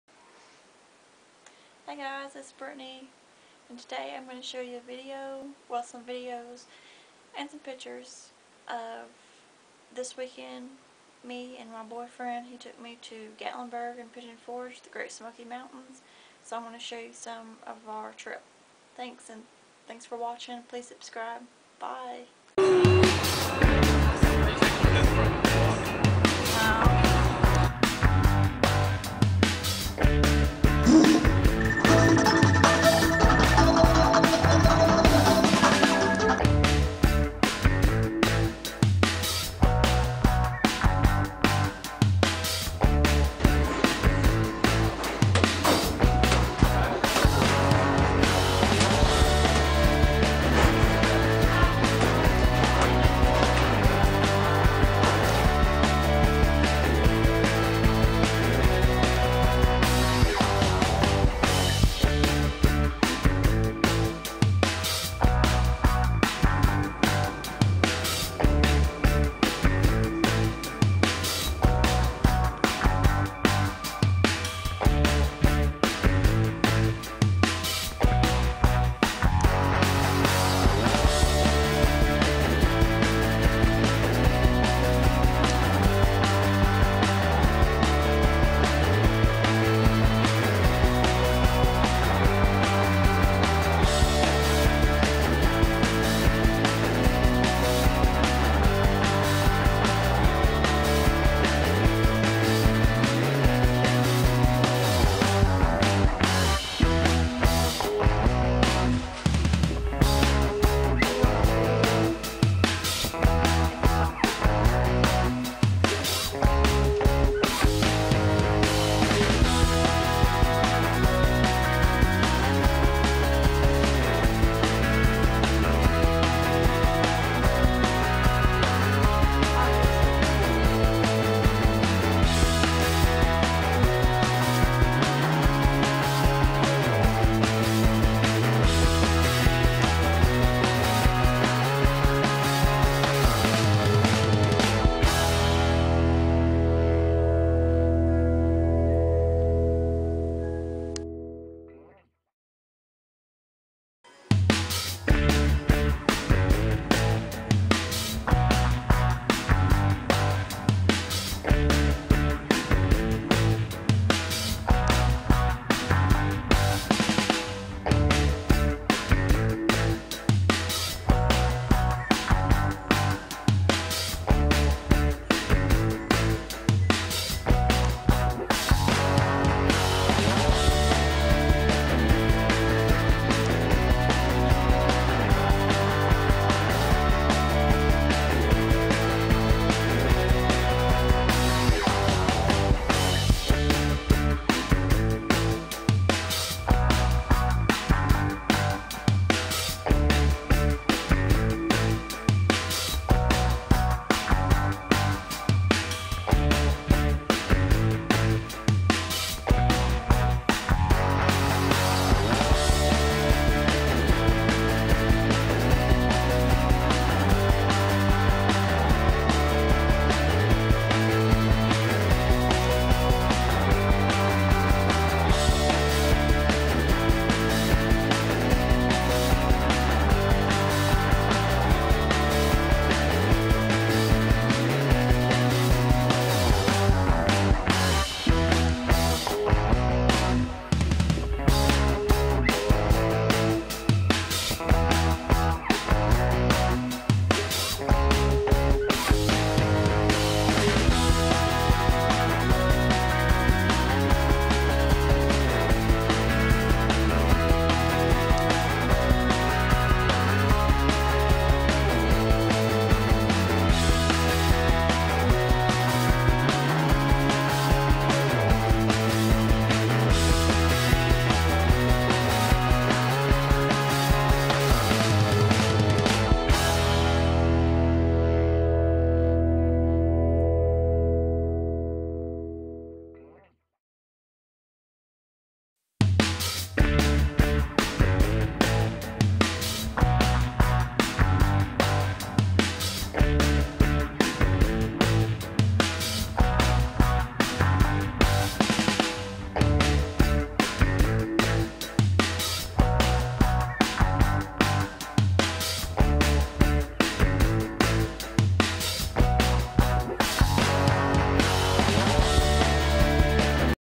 My boyfriend gave me the best trip ever!
my boyfriend toke me to the great smokey mountains its pigeon forge and Gatlinburg tennesse.Hope you enjoyed watching some of our trip be sure to like ,subscribe ,and comment video ideas  instagram brittneytyler12